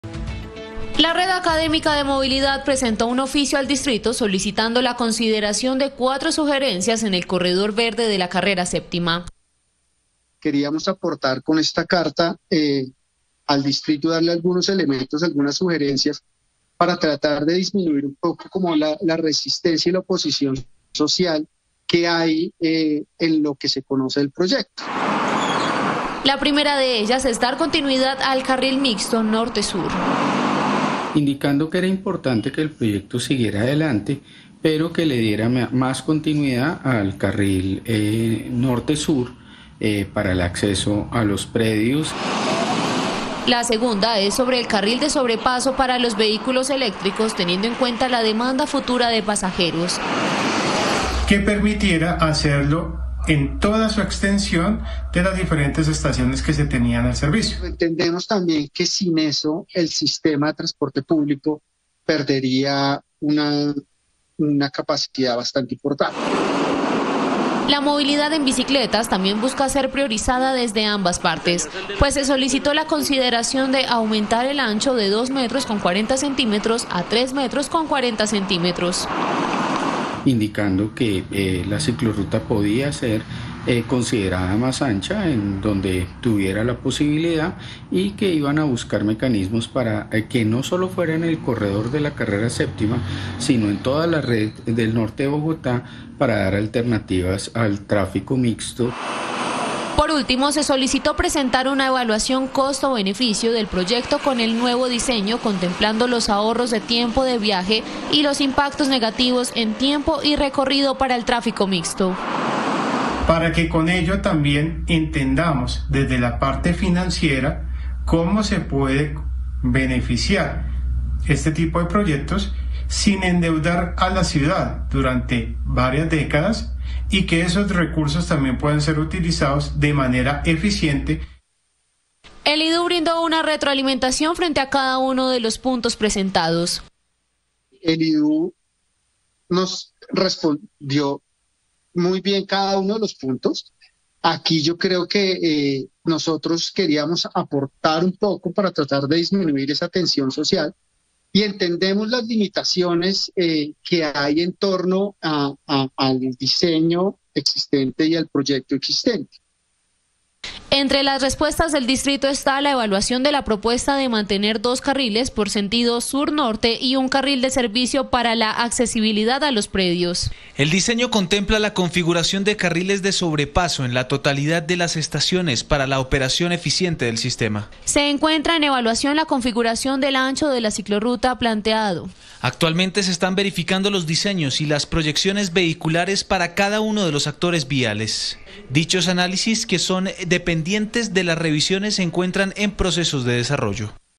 Piden Ajustar el ‘corredor verde’ de la Séptima | El Tiempo
La red académica de movilidad, compuesta por 40 docentes, envió un oficio al distrito y solicitó la revisión de los puntos clave sobre el corredor verde de la carrera séptima. Desde los ámbitos de accesibilidad, transporte público, ciclorrutas y económico, buscan modificaciones. El instituto de desarrollo urbano les respondió.